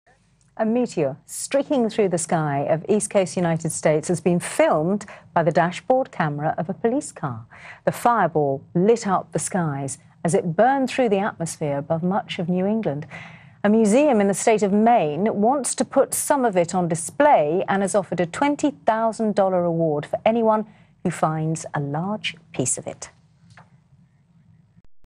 Shooting star lights up the night (USA) - 5 News - 18th May 2016
Another shooting star caught on a dashboard camera of a police car. Expect more of these as more cameras are recording more of our world and the Earth is being bombarded by millions of particles every day (most the size of a grain of sand thankfully).